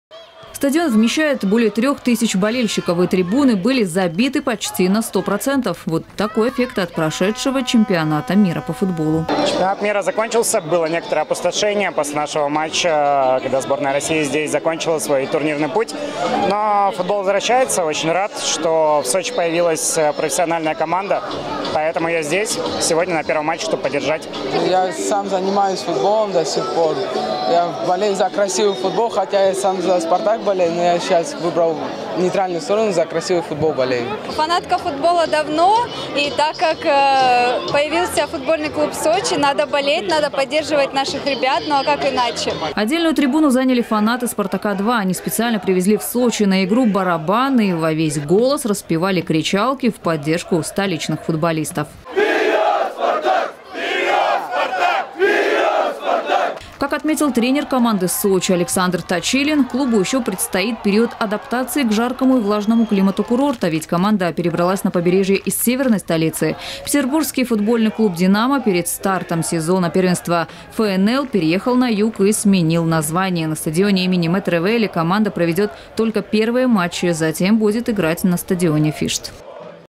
Со счётом 0:1 «Сочи» уступил «Спартаку-2» в матче первого тура ФНЛ. Новости Эфкате
Игра прошла накануне на стадионе имени Славы Метревели.

Стадион вмещает более 3 тысяч болельщиков. И трибуны были забиты почти на сто процентов. Отдельную трибуну заняли фанаты «Спартака-2», они специально привезли в Сочи на игру барабан и во весь голос распевали кричалки в поддержку столичных футболистов.

Как отметил тренер команды «Сочи» Александр Точилин, клубу ещё предстоит период адаптации к жаркому и влажному климату курорта, ведь команда перебралась на побережье из северной столицы. Петербургский футбольный клуб «Динамо» перед стартом сезона первенства ФНЛ переехал на юг и сменил название.

На стадионе имени Метревели команда проведёт только первые матчи, затем будет играть на стадионе «Фишт».